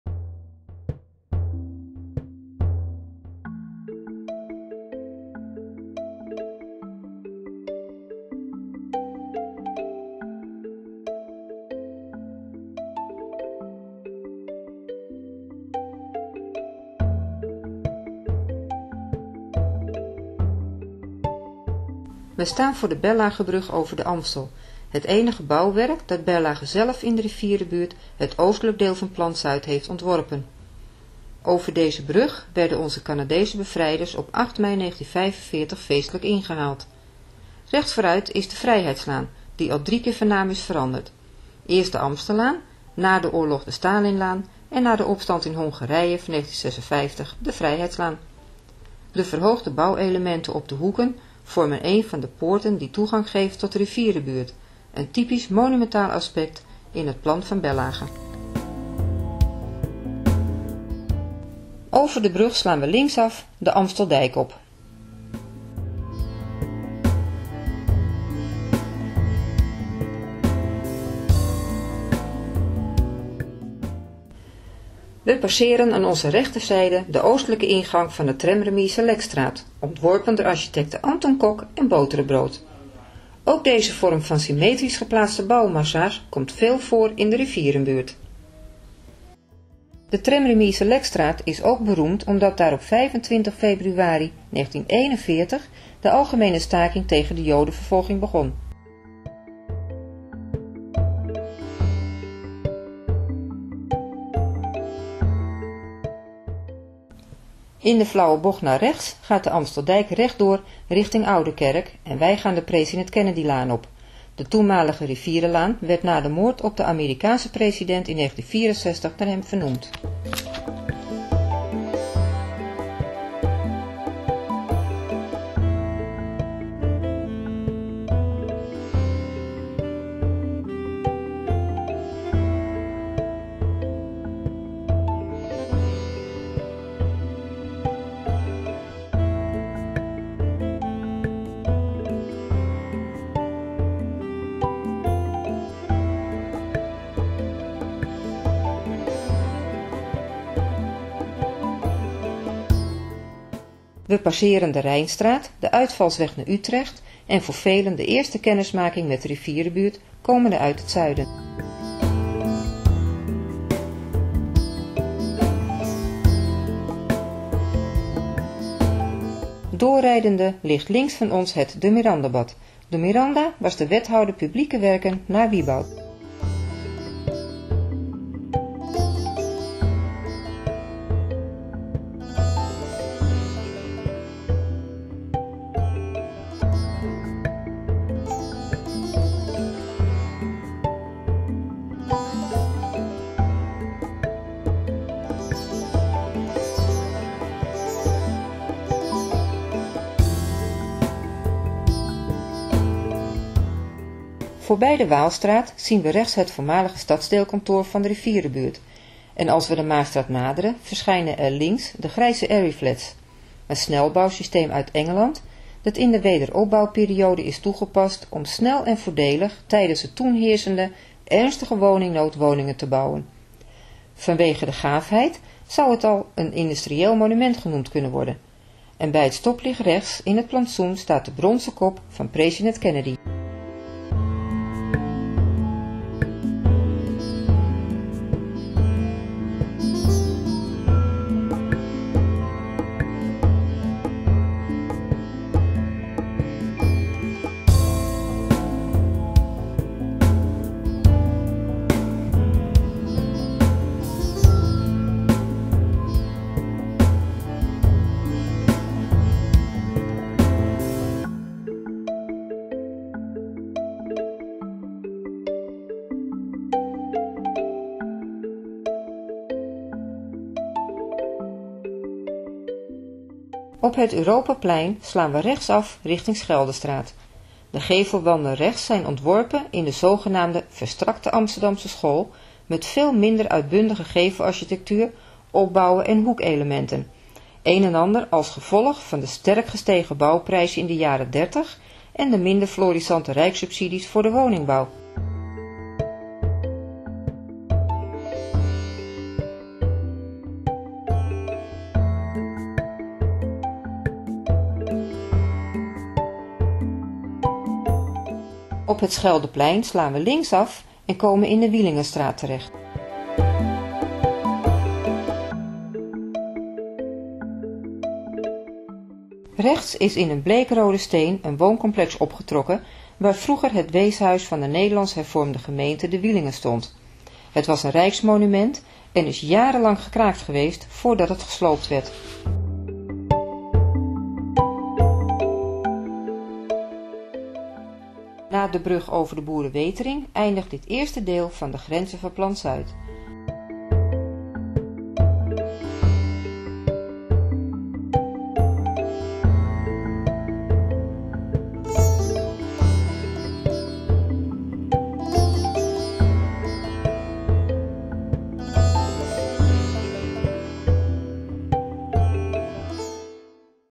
De grenzen van Plan Zuid (1)
Van Berlagebrug tot Diepenbrockstraat.
Een beeldproject (deel 1) van Stiching Geheugen van Plan Zuid
Commentaarstem: Jenny Wiersema-Edens